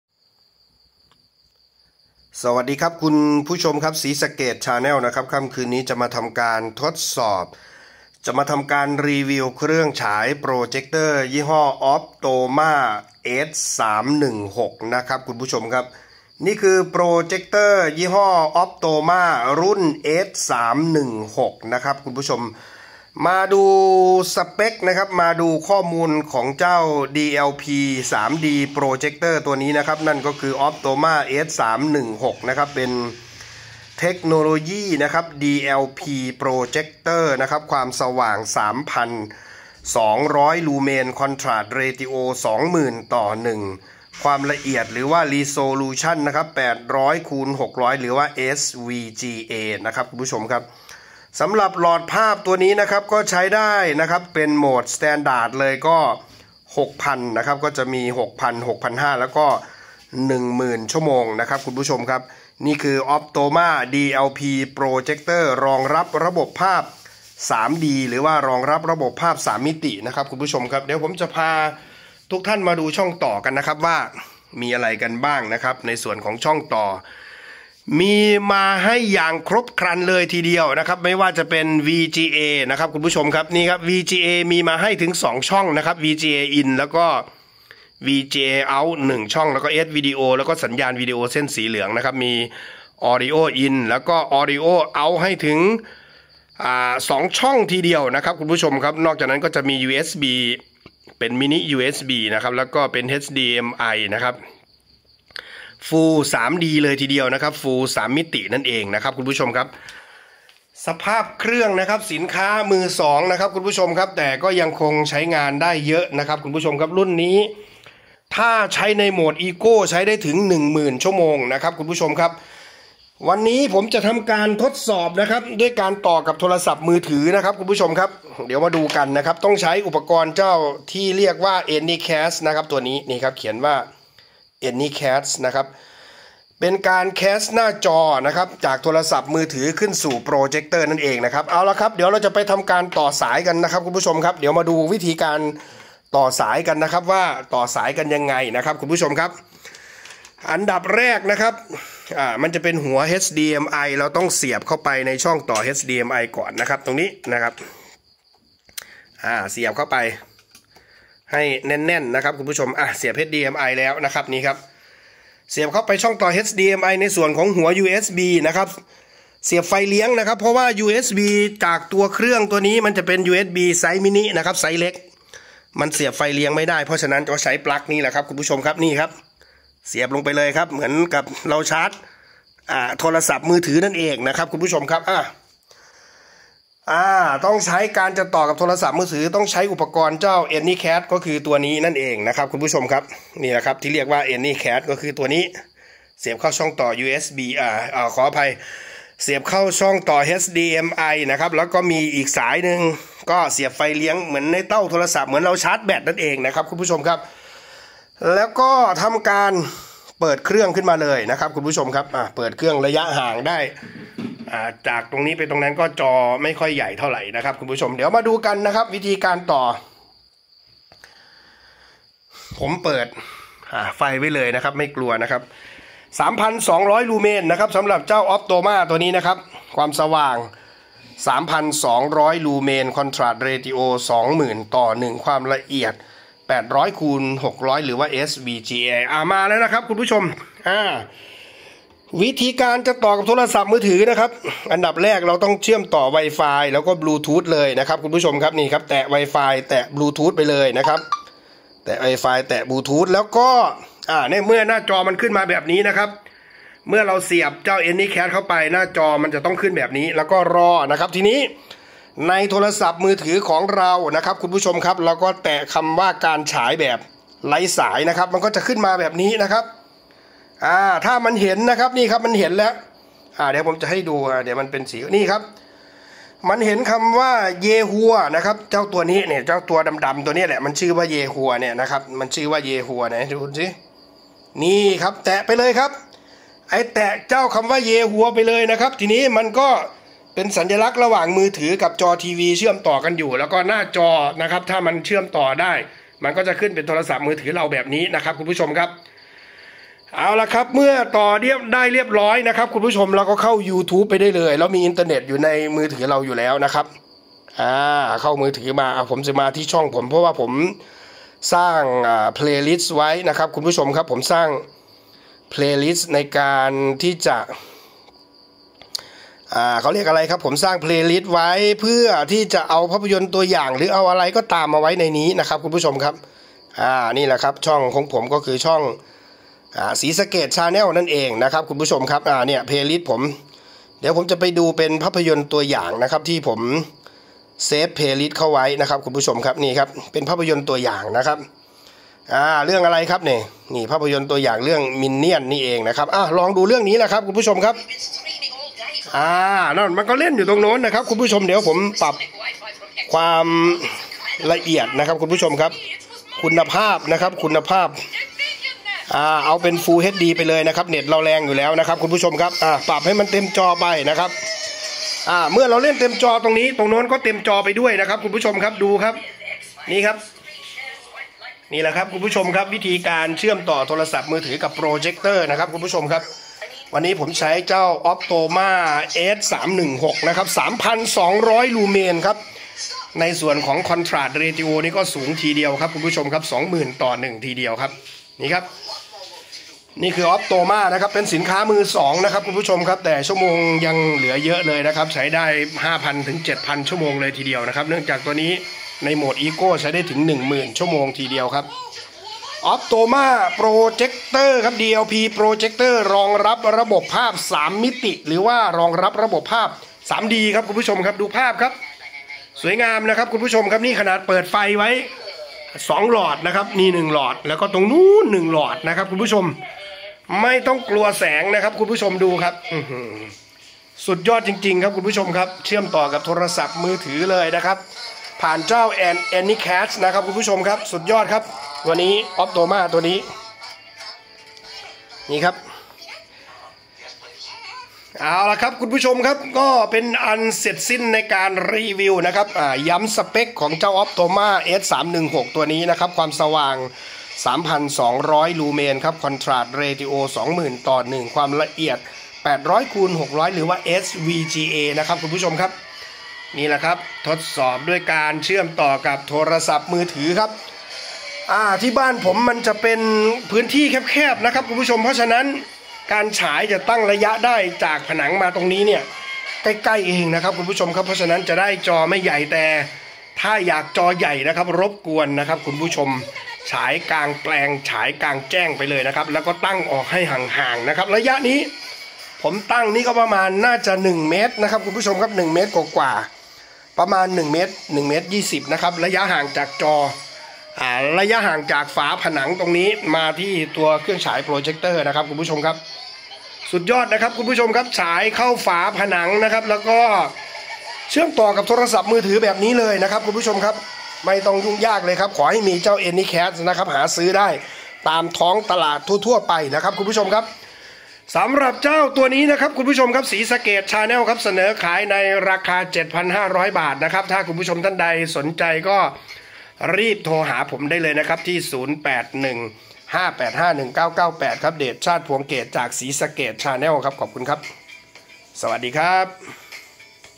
ขายโปรเจคเตอร์มือ2 Optoma S316 Optoma Projector ขายโปรเจคเตอร์ โปรเจคเตอร์มือสอง แสง 3200 ลูเมนส์
ขายโปรเจคเตอร์มือ2 Optoma S316 Optoma Projector ขายโปรเจคเตอร์ โปรเจคเตอร์มือสอง ความสว่าง 3200 ลูเมนส์ Contrast 20000:1 ความละเอียด 800x600 SVGA ขาย 7500 บาท โทร